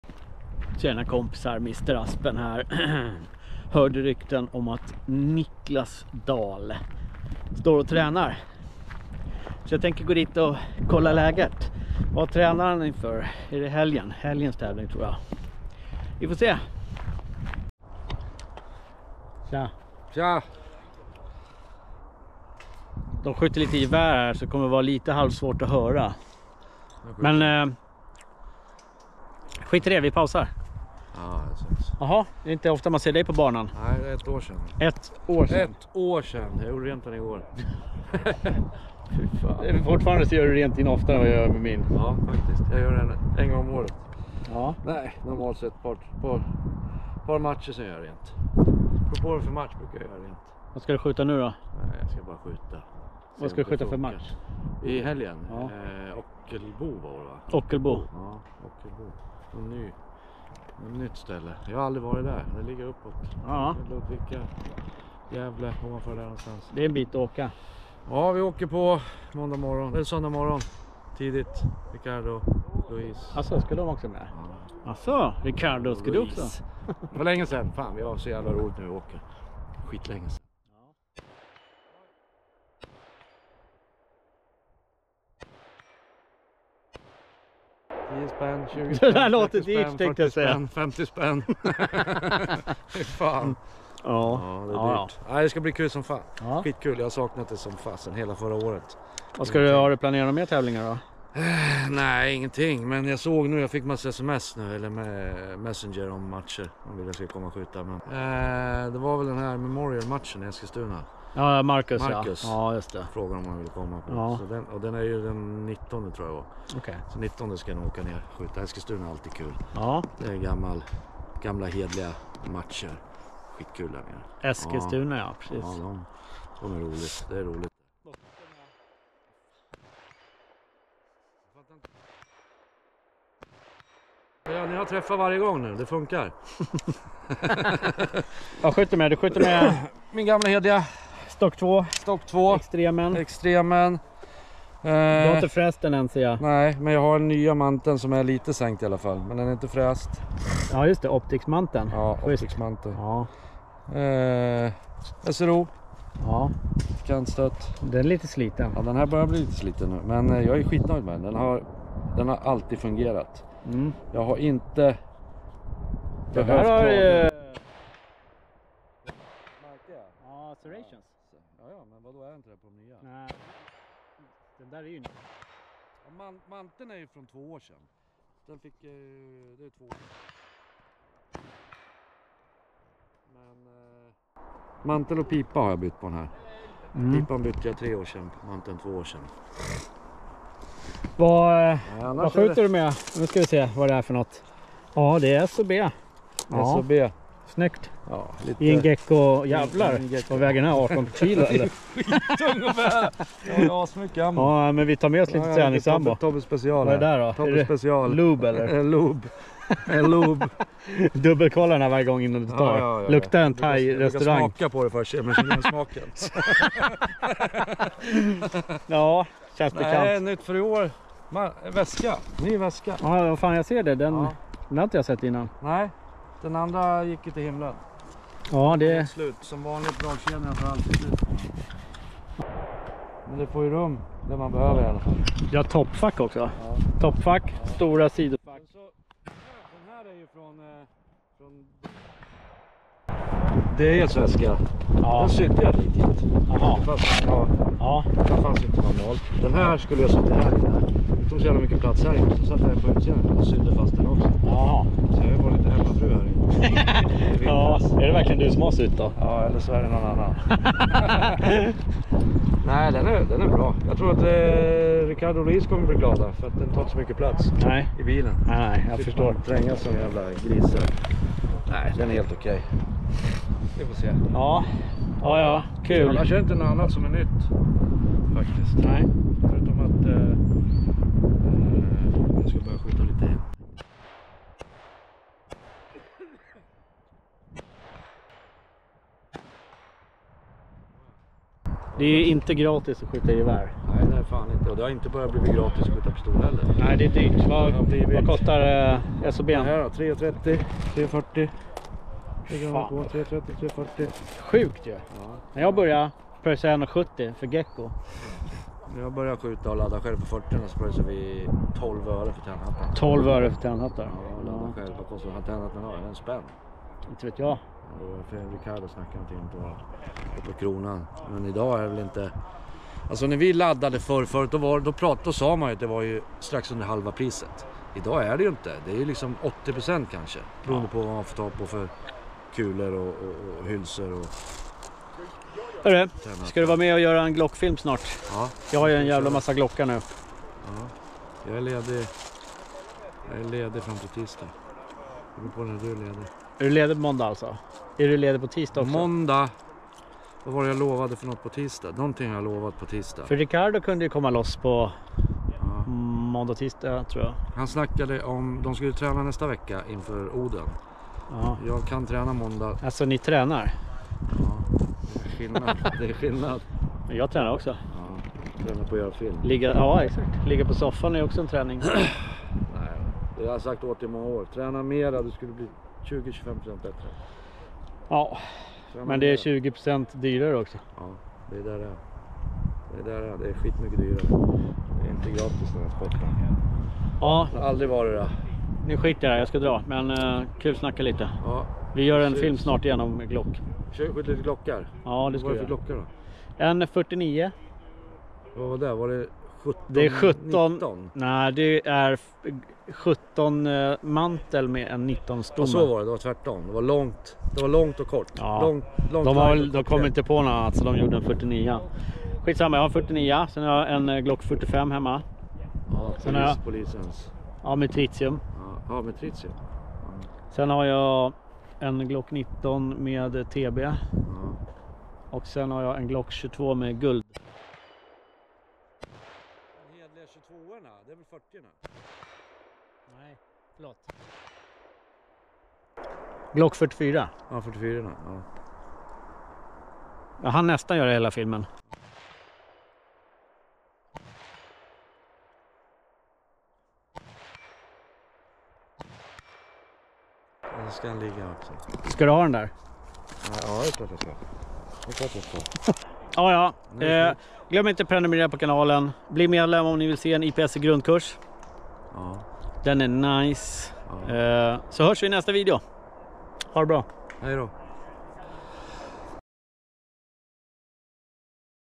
Niclas Dal Återkomsten! (Team DAE)
Niclas Dal smyger tillbaka till IPSC efter en liten period med gubbkrämpor. 😎❤️